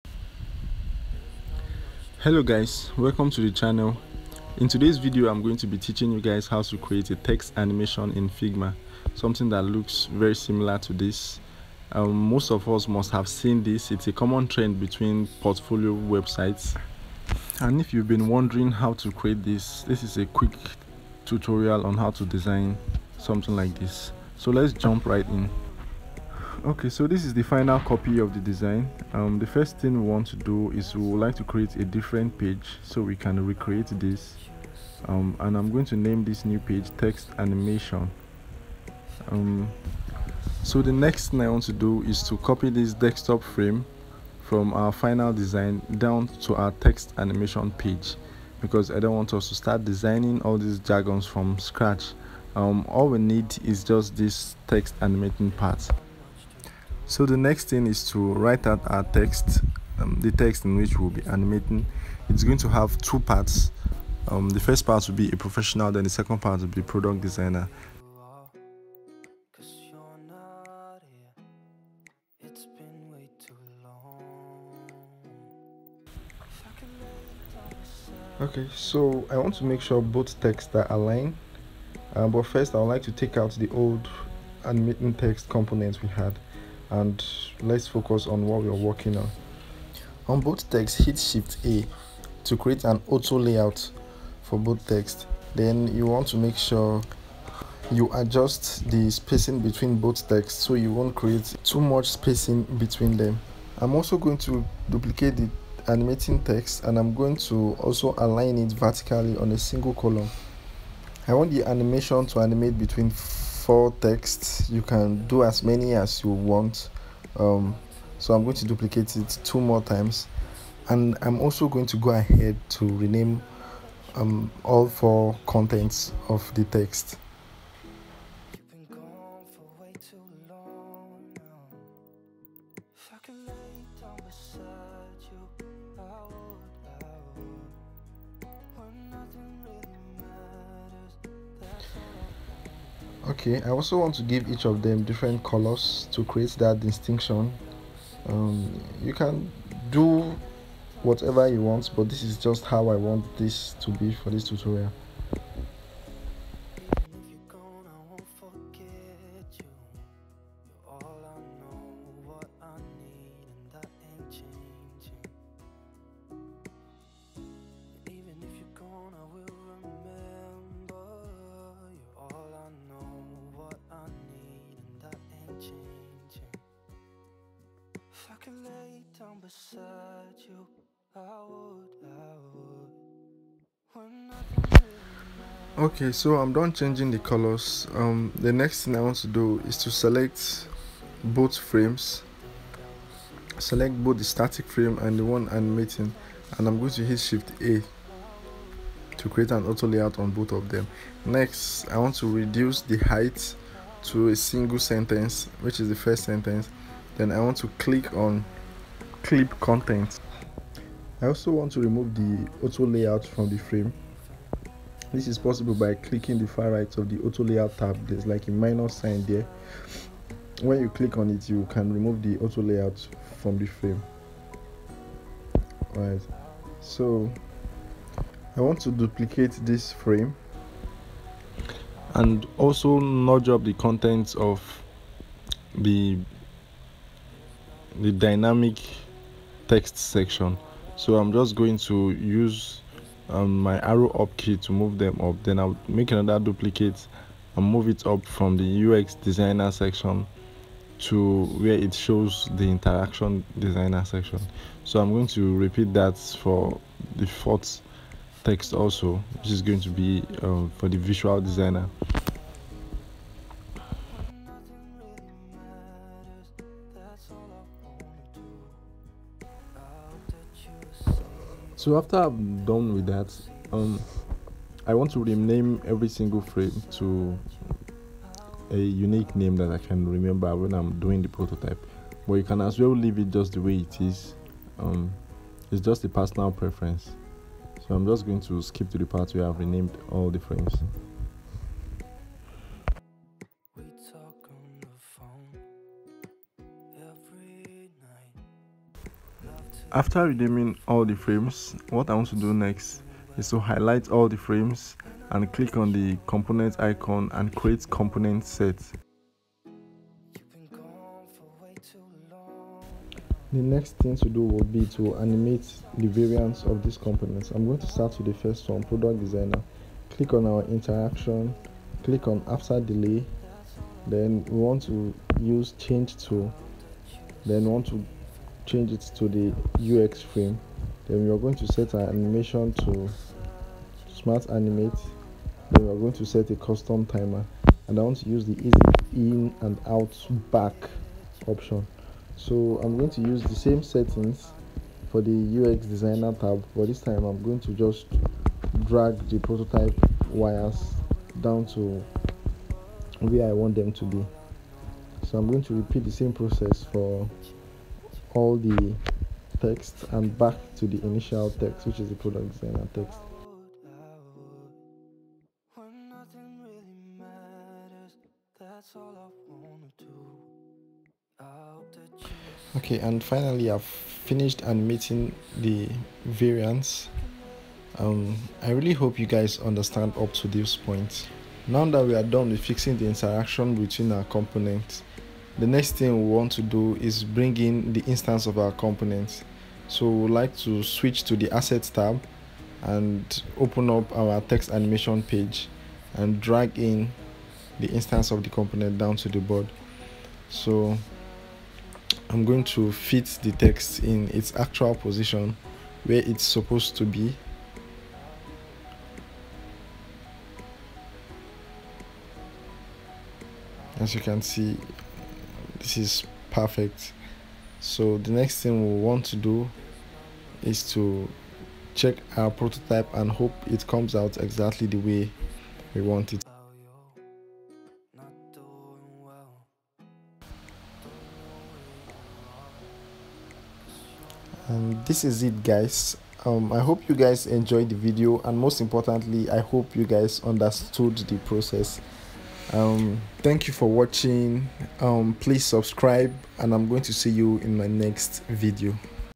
Figma Text Animation: Create an animating text in figma, Learn about variants and prototypes
Common sliding text animation in Figma.